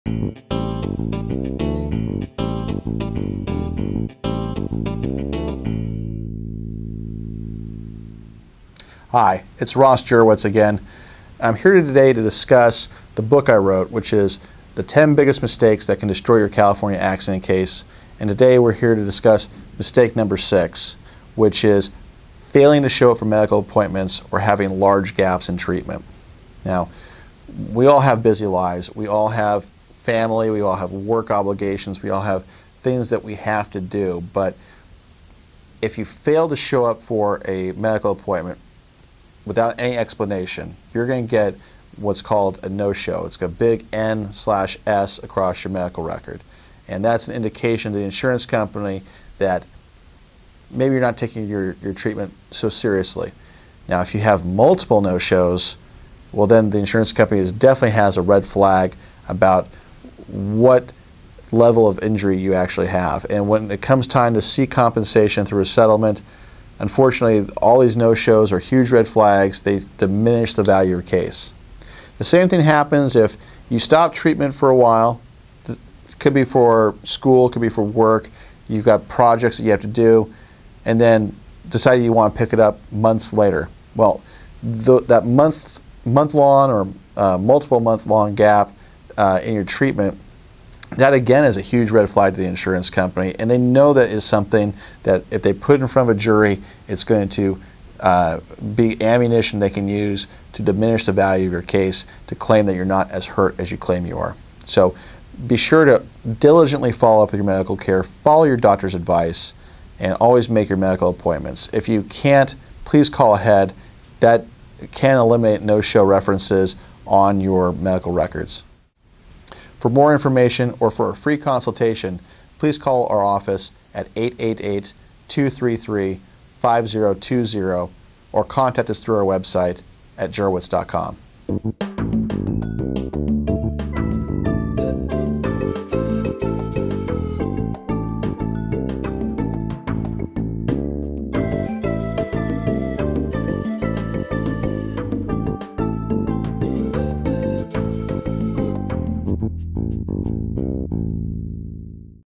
Biggest CA Personal Injury Mistake #6: No Show at Medical Appointments
– San Diego injury attorney Ross Jurewitz of the Jurewitz Law Group, a California personal injury law firm, discusses his free book, The Ten Biggest Mistakes that can Destroy Your California Accident Case, and here discusses Mistake #6: failing to show up to medical appointments with your doctors. One or even two "no show" appointments will not ruin your personal injury claim.  However, a repeated number of failed appointments is ammunition for the insurance company to argue that you are not hurt at all or not hurt as much as you claim, thereby reducing the value of your personal injury case.